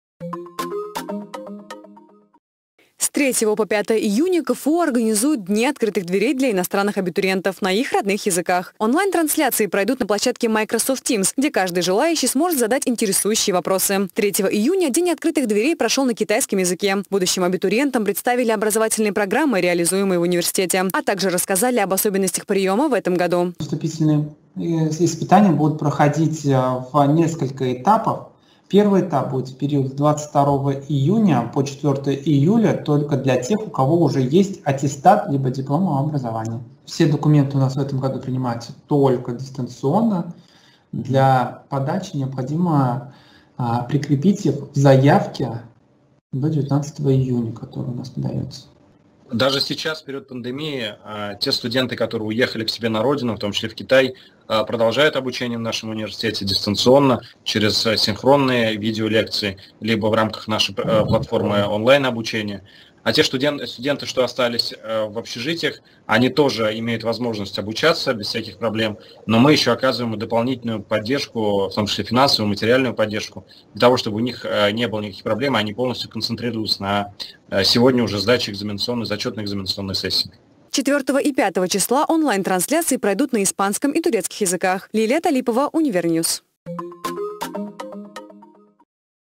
ДЕНЬ ОТКРЫТЫХ ДВЕРЕЙ КФУ
Пандемия коронавируса, охватившая мир, внесла коррективы в приемную кампанию российских вузов. Учебные учреждения проводят дни открытых дверей с помощью прямых эфиров и решают, как дистанционно организовать вступительные испытания для абитуриентов. В Казанском федеральном университете дни открытых дверей также проходят в онлайн формате. К прямой трансляции можно подключится из любой точки мира с помощью площадки Microsoft Teams.